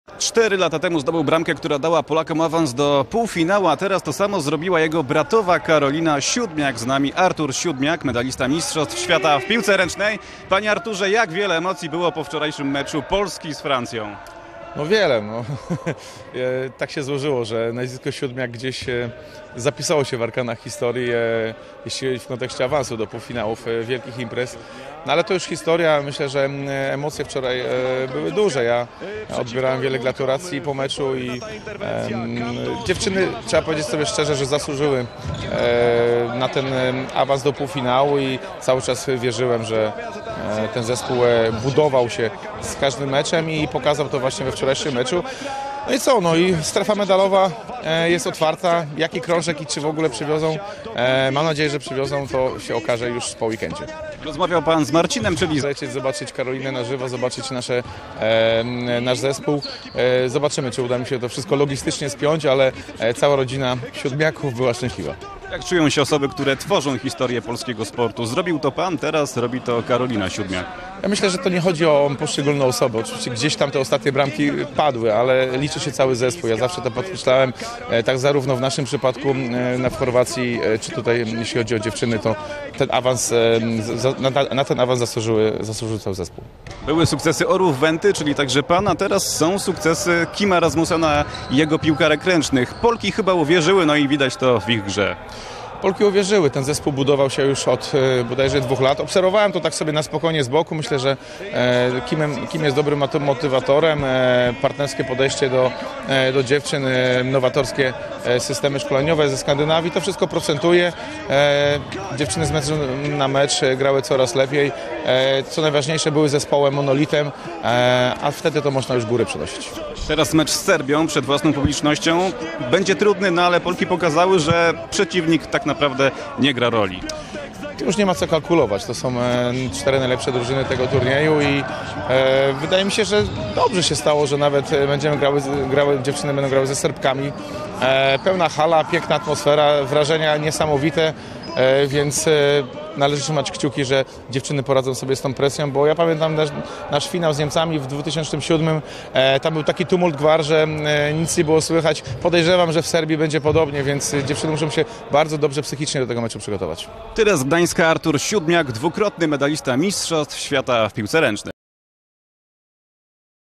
Artur Siódmiak o awansie Polek: cała rodzina oszalała
Artur Siódmiak, srebrny (2007) i brązowy (2009) medalista MŚ, ocenił postawę polskich szczypiornistek w Serbii. W drużynie biało-czerwonych gra jego szwagierka - Karolina.